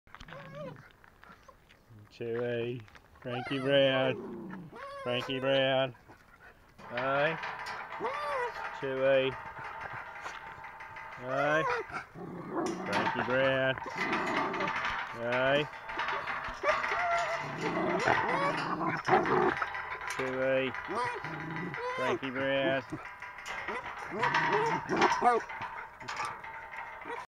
Miniature Schnauzer
Chewie and Frankie Brown ,Schnauzers ,6 Months Old Males ,Salt and pepper and a Tan